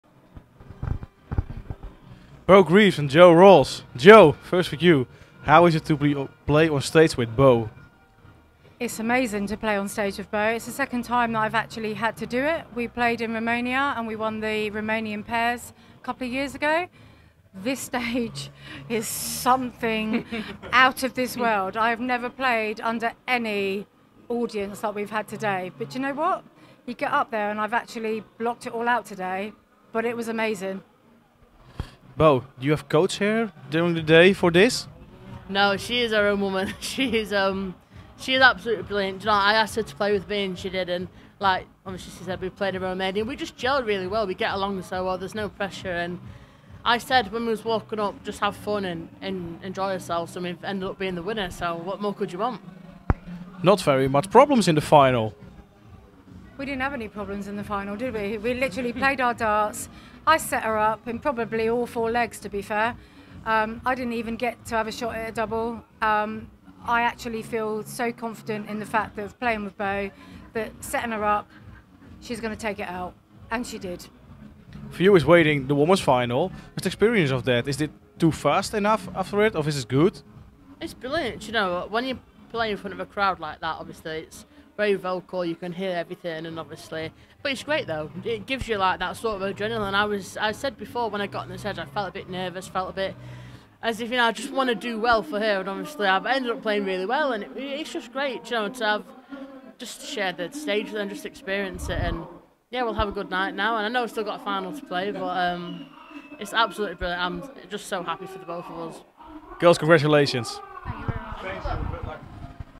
Beau Greaves & Jo Rolls: 'This stage is something out of this world' | Dutch Open 2024
In het weekend van 1 t/m 4 februari werd de 51ste van TOTO Dutch Open Darts georganiseerd in de Bonte Wever, te Assen. Zondag stonden de finales op het programma. Beau Greaves en Jo Rolls grepen de titel in het dames koppeltoernooi door Lorraine Winstanley en Paige Pauling met 4-0 in de finale te verslaan. Na afloop sprak Darts Actueel met beide dames.

Al het dartnieuws waaronder de TOTO Dutch Open Darts is terug te vinden op onze website!